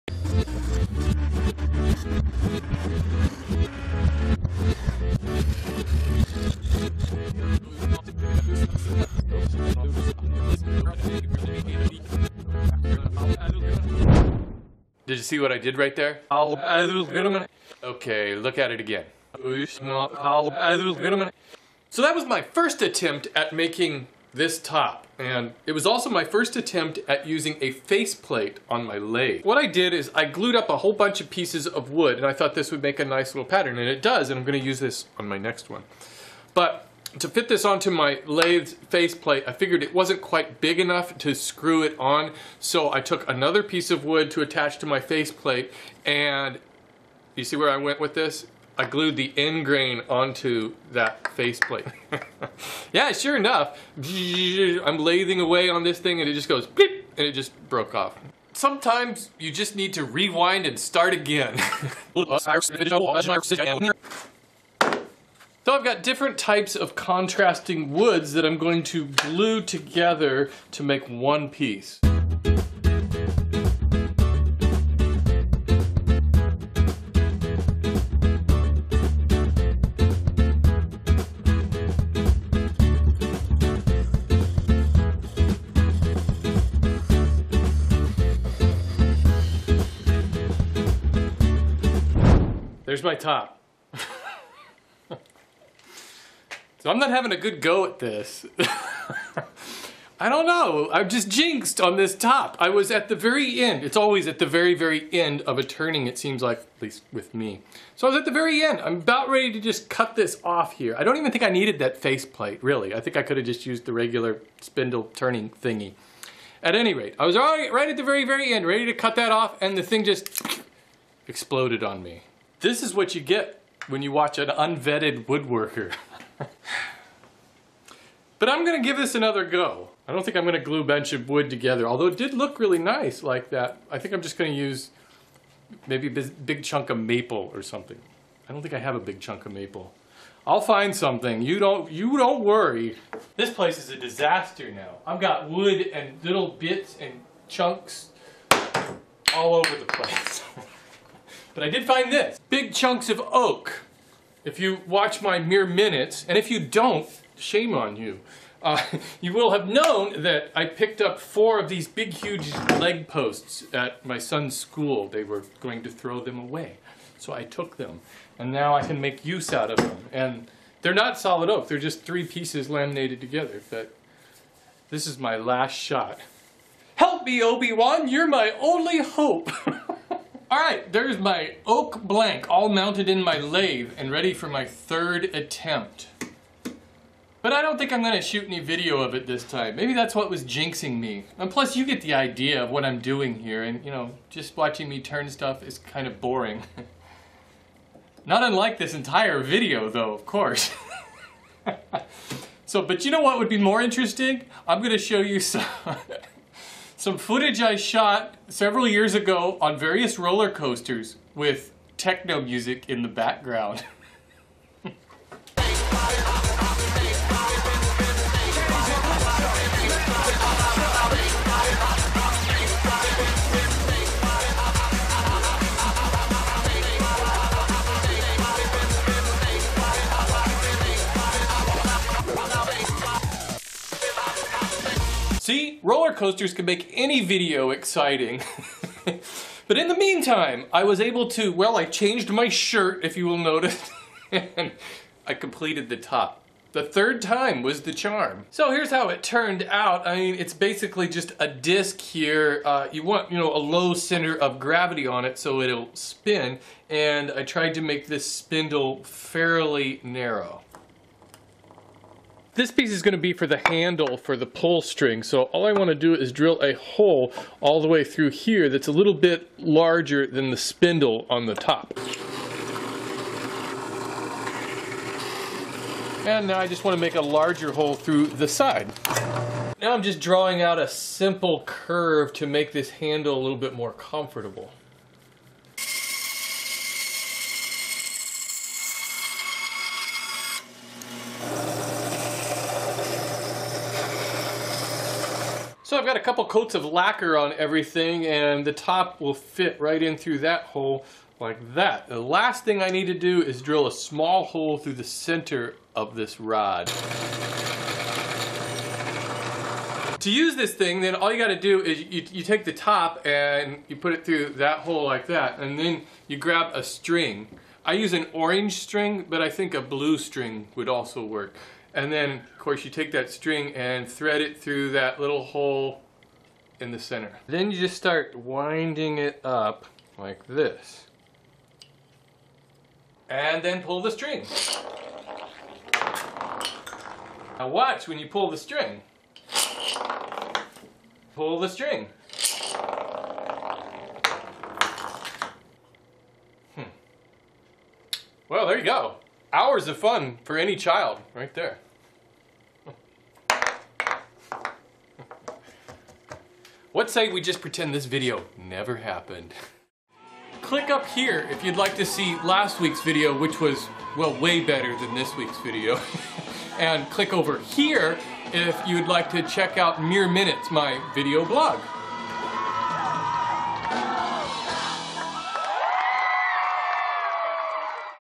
Make a spinning (sorta) toy (kinda) top (well, okay)
Had a bit of trouble making the simplest of projects this week. This is a spinning toy top. It really should be quite simple to turn on a lathe and I have no doubt most normal people could make this in about an hour. It took me three attempts.

MAILING ADDRESS:

WWMM
Category
Howto & Style